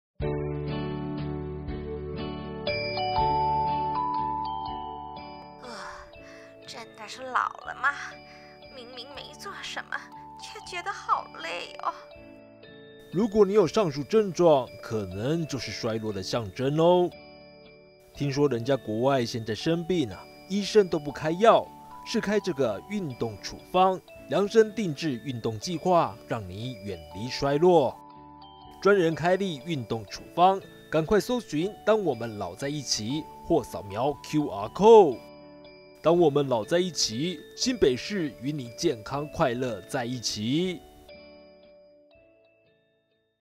什麼是新北市「運動處方」？懶人包告訴你！
藉由量測6公尺步行速度及手部握力檢測開立運動處方，設計專屬運動計畫，達到以運動介入有效反轉衰弱及預防失能。